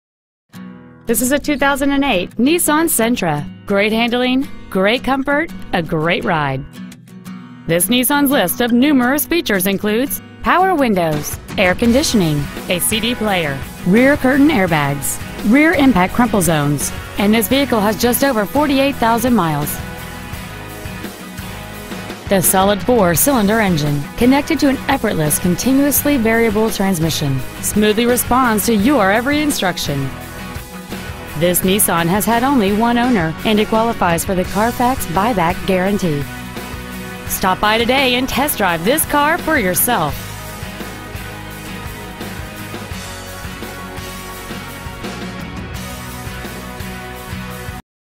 Used 2008 Nissan Sentra Decatur GA
Year: 2008
 Make: Nissan
 Model: Sentra
 Engine: Gas I4 2.0L/122
 Trans.: Variable
 Exterior: Sonoma Sunset Pearl
 Miles: 48,520
 Interior: Charcoal/steel

 SUPERIOR CHEVROLET

 http://

 2008 Nissan Sentra Decatur GA